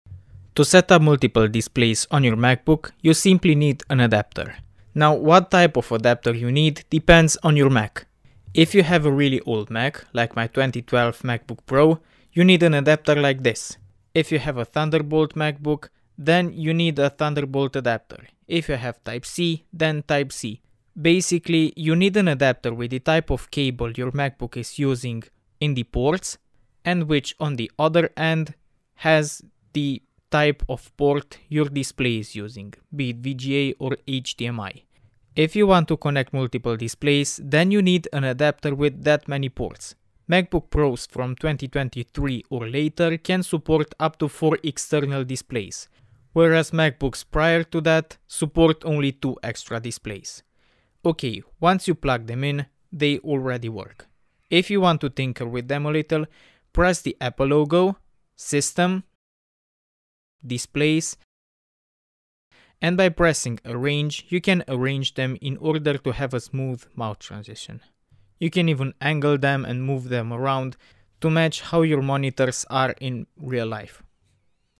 How to setup multiple displays on Macbook
To setup multiple displays on your macbook you simply need an adapter. Now it depends on the mac we are talking about. If you have a really old mac, like my 2012 macbook pro, you need an adapter like this, if you have a thunderbolt macbook, then you need a thunderbolt adapter, if you have type C, then Type C.
Basically, you need an adapter with the type of cable your macbook is using on one end, and a cable your display is using on the other end. Be it VGA or HDMI.
If you want to connect multiple displays, then you need an adapter with that many ports. Macbook Pros from 2023 or later can support up to 4 external displays, whereas macbooks prior to that suppiort only two extra displays.

Okay. Once you plug them in, they already work. If you want to tinker with them a little, press the apple logo, System, Displays, and by pressing arrange you can arrange them in order to have a smooth mouse transition. That way you can make the mouse go as you have your monitors set.